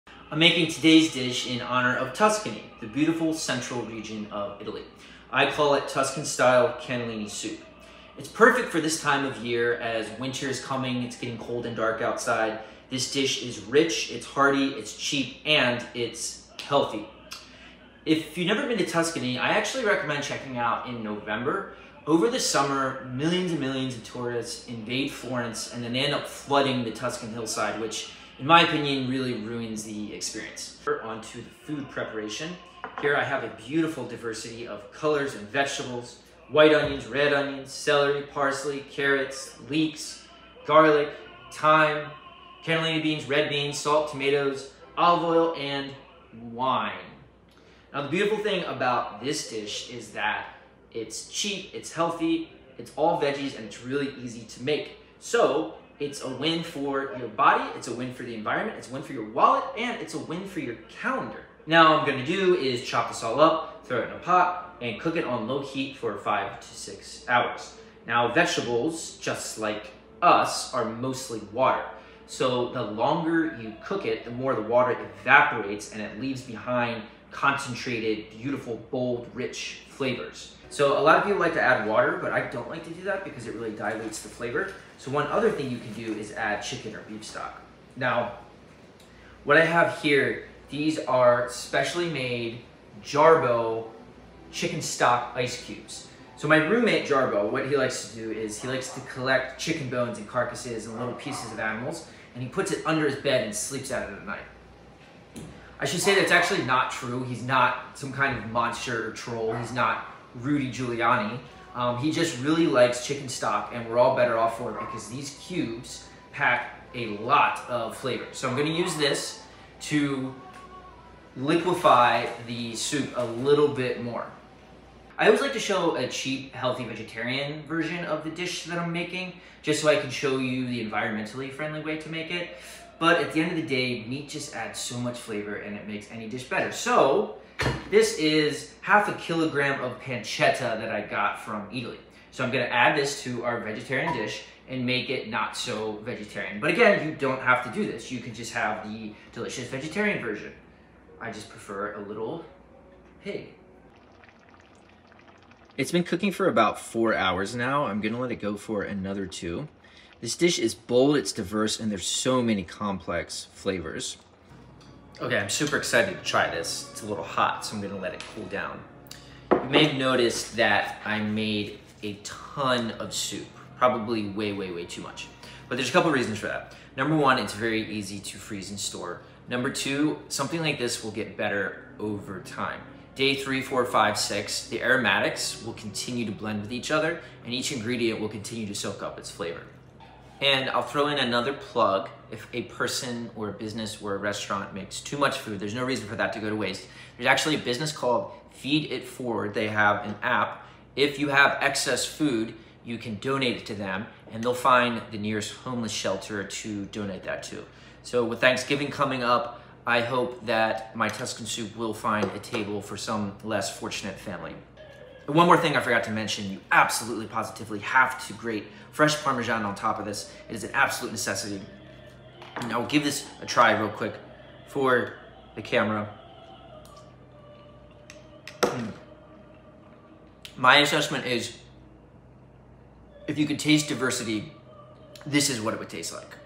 Tuscan Style Canellini Soup
This Tuscan Style soup is the perfect dish for late fall and early winter.  It's rich, it's bold and it's packed with a diversity of flavors.  Eating this will will transport you to a beautiful hillside in the heart of Tuscany.